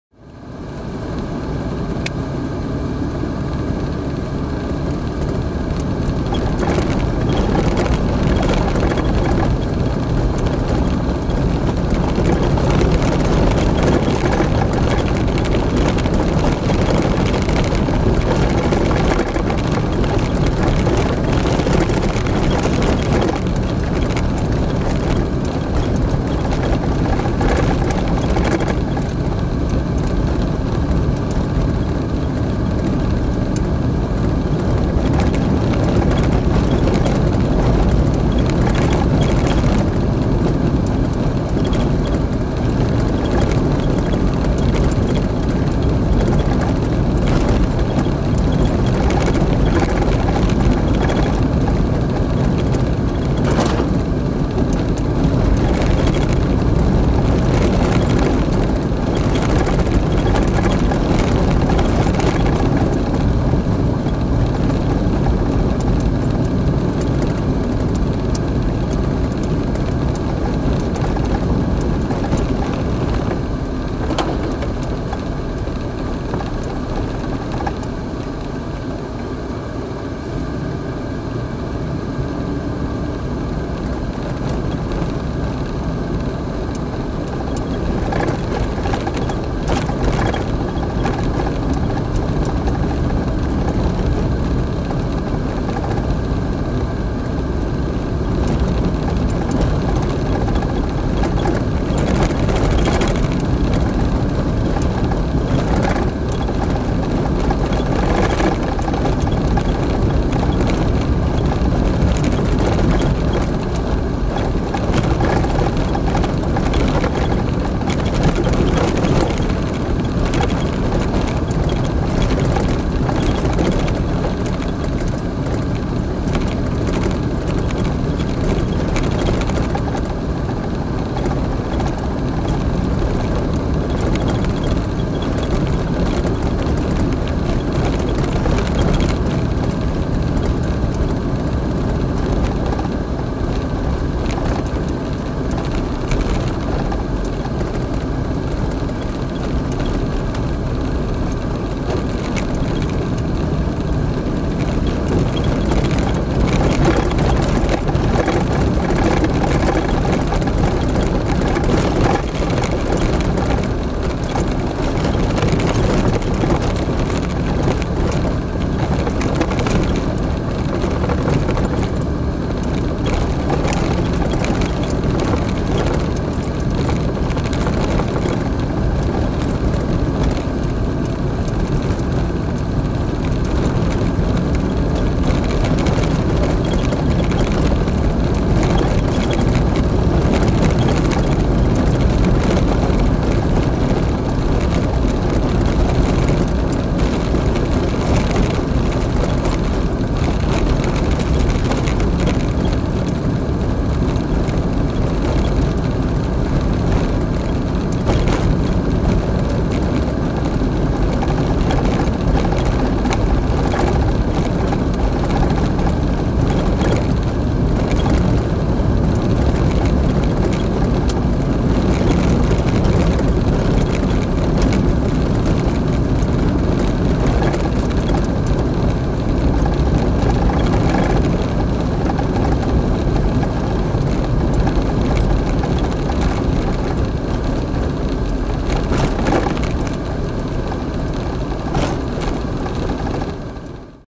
Quitman Pass Road, Texas
Driving Quitman Pass Road, Texas
On my Zebulon Montgomery Pike Trail Mexico